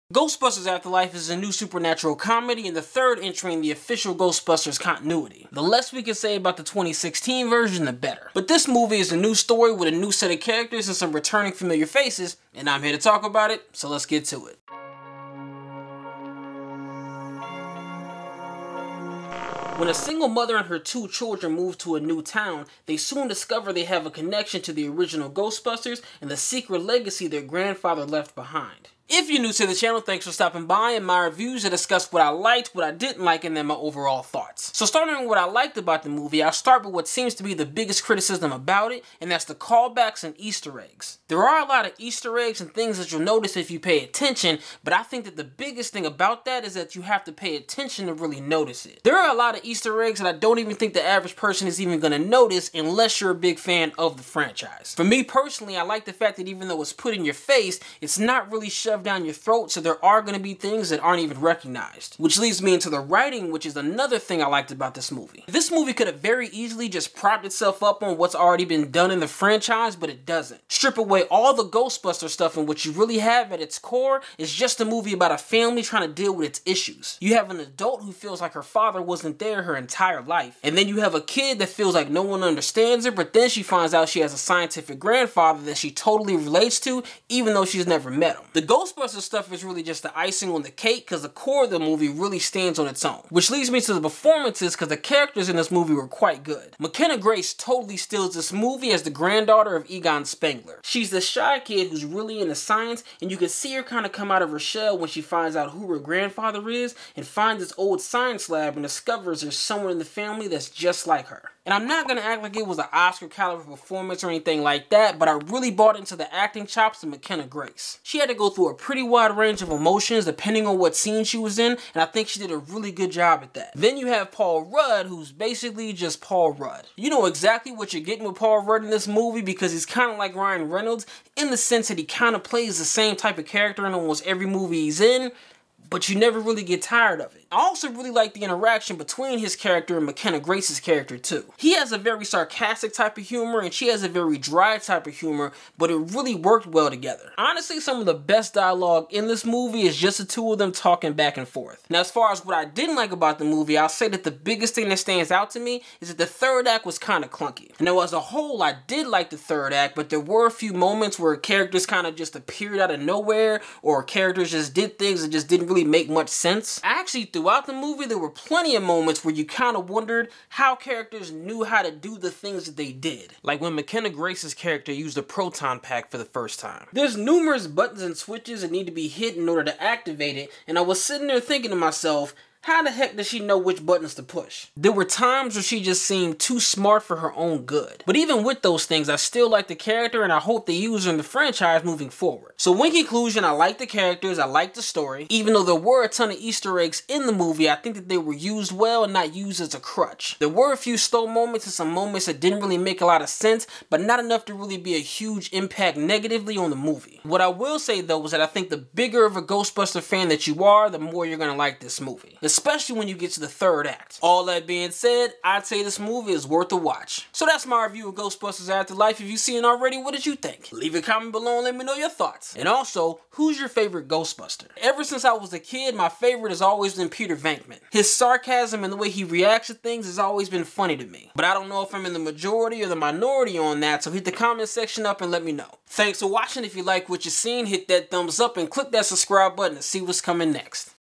Ghostbusters: Afterlife Movie Review
When a single mother and her two children move to a new town, they soon discover they have a connection to the original Ghostbusters and the secret legacy their grandfather left behind. Here's Johnnie Omega's review of Ghostbusters: Afterlife!!!